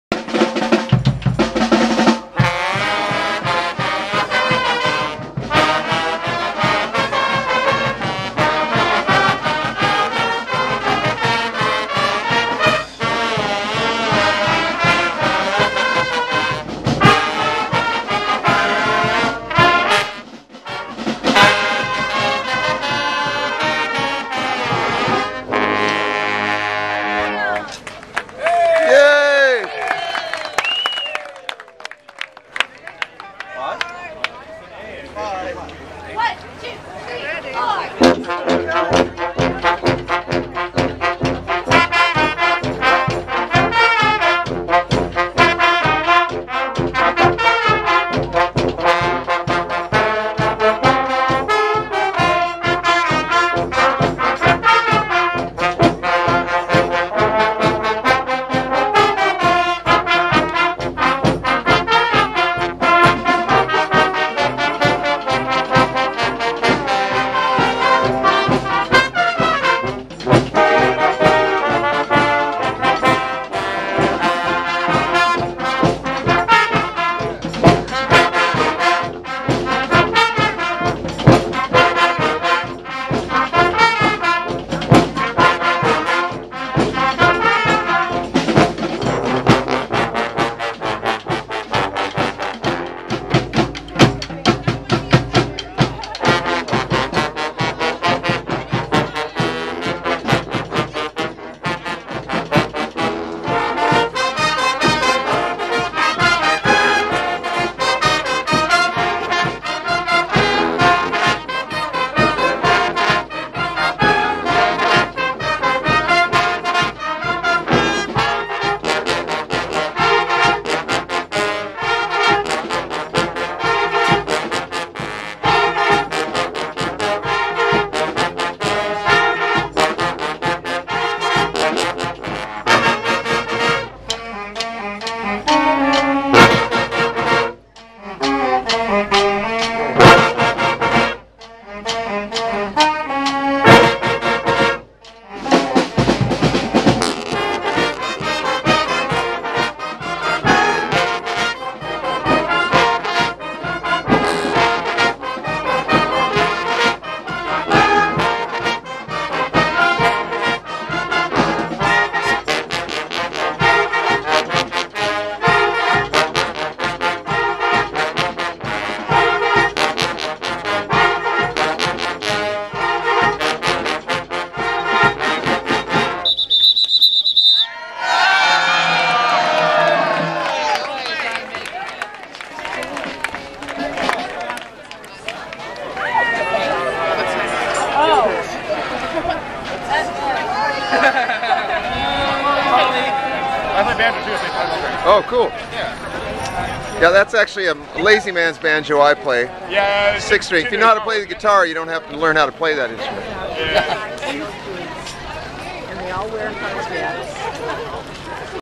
HSU Marching Lumberjacks at the Soroptimist Crafts Fair - September 4, 2011 Fort Bragg CA
HSU Marching Lumberjacks band at the Paul Bunyan Days Soroptimist Crafts Fair - September 4, 2011 Fort Bragg CA